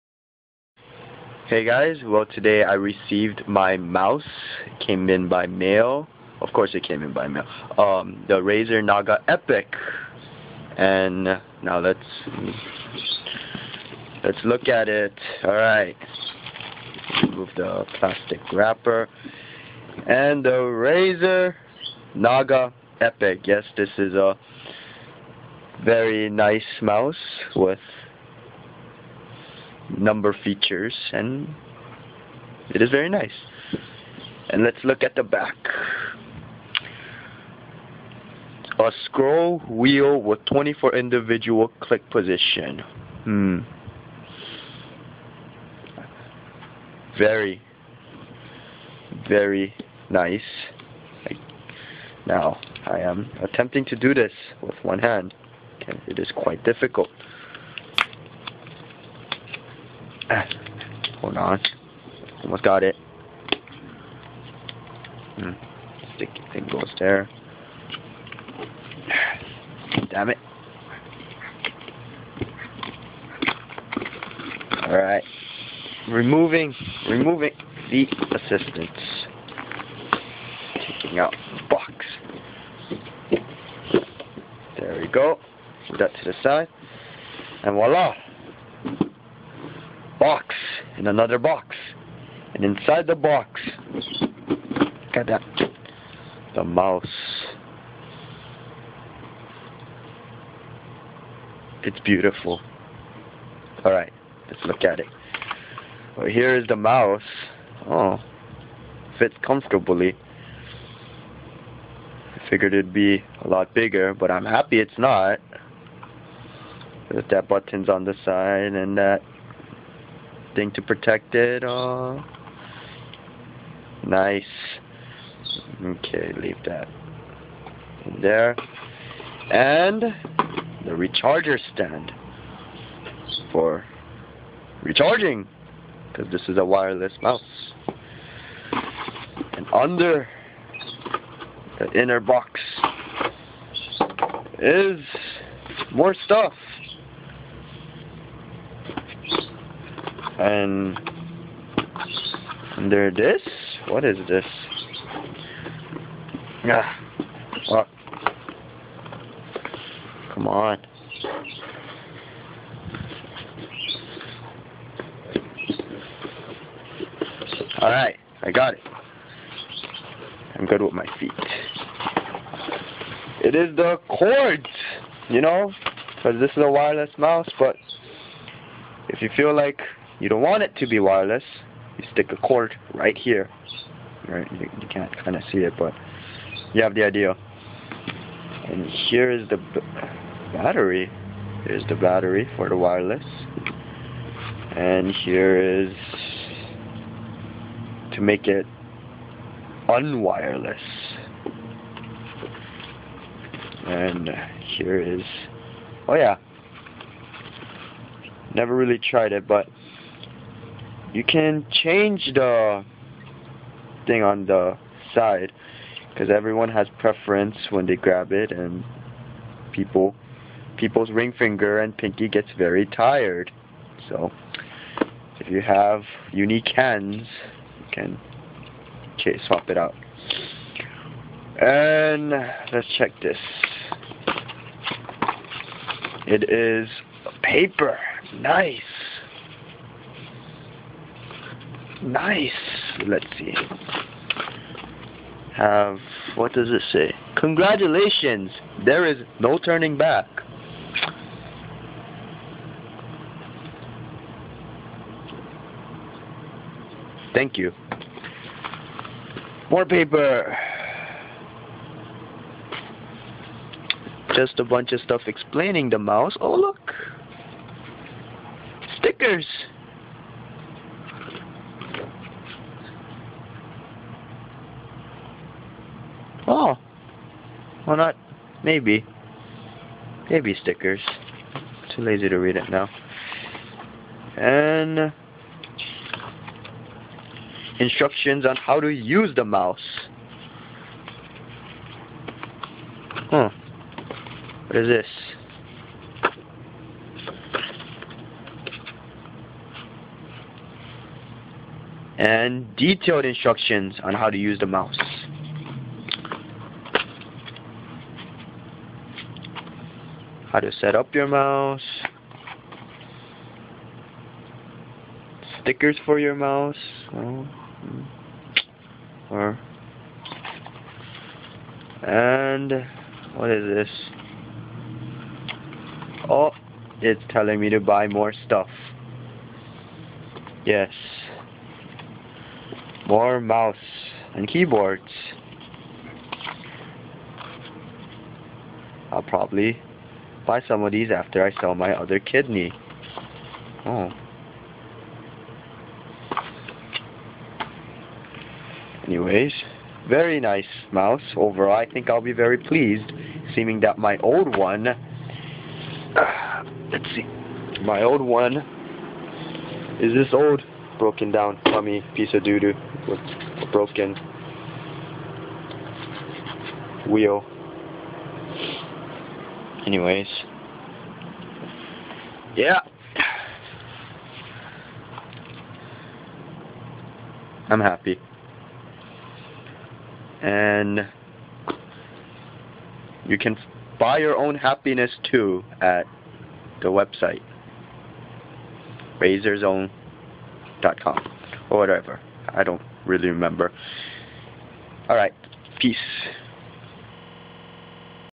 Unboxing of my new mouse
I unbox my new mouse.